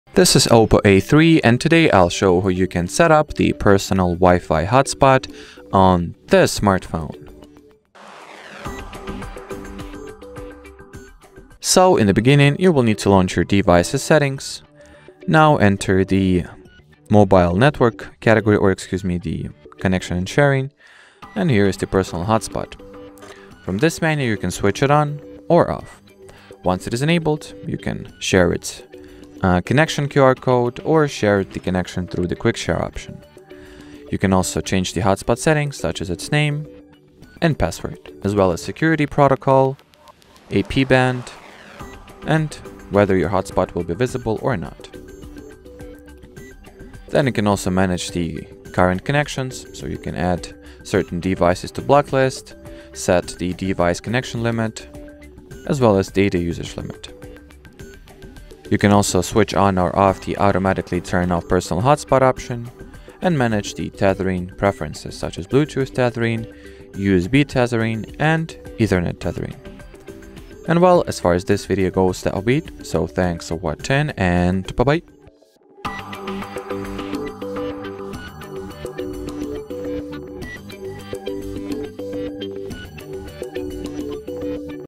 OPPO A3 - How to Enable Personal Hotspot | Share Your Internet Connection
Hi! Need to share your internet connection with other devices? In this video, we’ll show you how to enable Personal Hotspot on your OPPO A3, turning your phone into a mobile Wi-Fi hub.

How to enable Personal Hotspot on OPPO A3?
How to share your internet connection using OPPO A3?
How to turn your OPPO A3 into a mobile Wi-Fi hub?